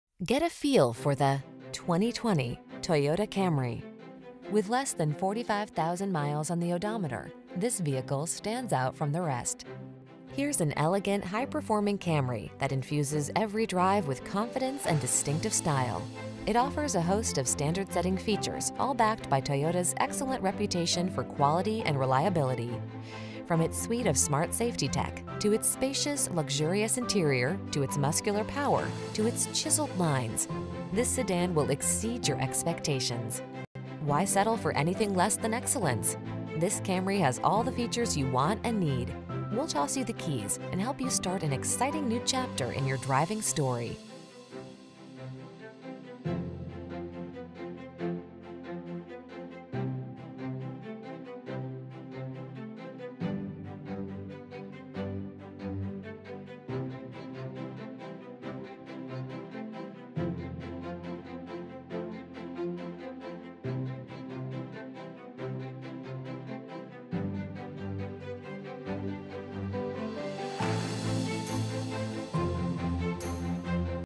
2020 Toyota Camry Miami, Miami Beach, Homestead, Kendall, Coral Gables 65034
Gray Used 2020 Toyota Camry XLE available in 1740 SW 1st Street, Miami, Florida at Ferco Motors. Servicing the Miami, Miami Beach, Homestead, Kendall, Coral Gables, FL area.

2020 Toyota Camry XLE - Stock#: 65034

Ferco Motors
1740 SW 1st Street